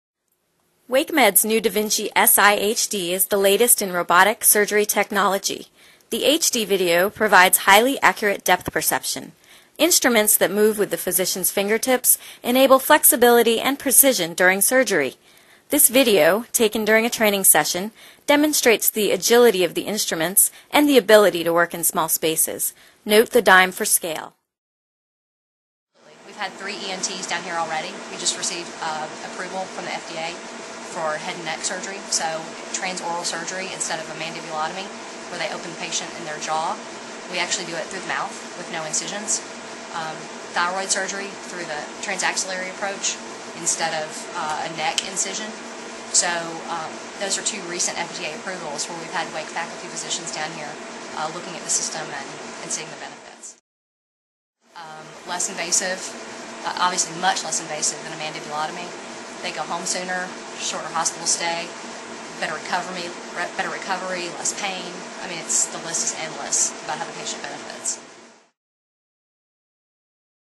WakeMed's DaVinci Si with 3D HD vision (robotic surgery)
The da Vinci Si with 3D HD vision is the most advanced platform for minimally invasive surgery available today.

It replaces an older model of the da Vinci Surgical System that was used by physicians of Wake Specialty Physicians Urology and OBGYN for the past five years at WakeMed Health & Hospitals in Raleigh, NC.